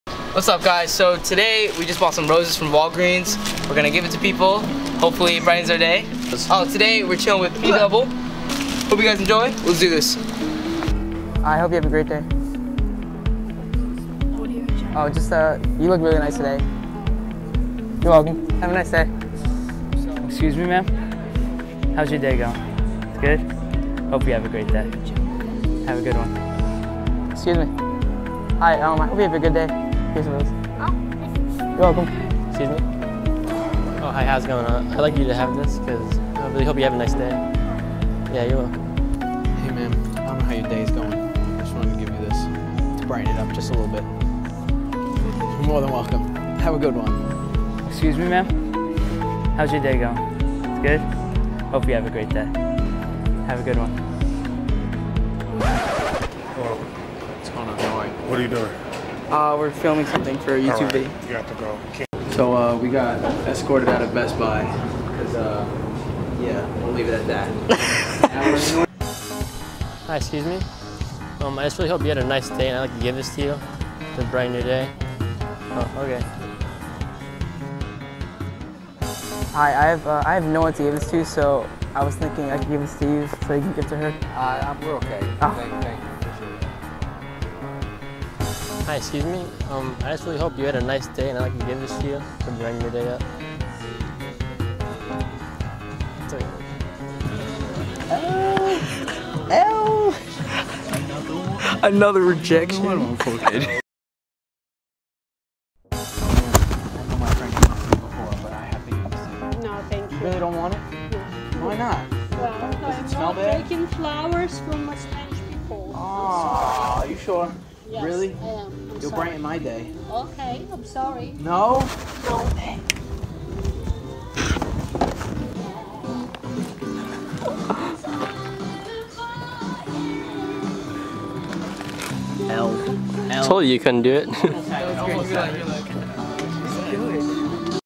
Giving Strangers Roses Experiment
We wanted to make people smile... so we bought some roses, and made people smile :)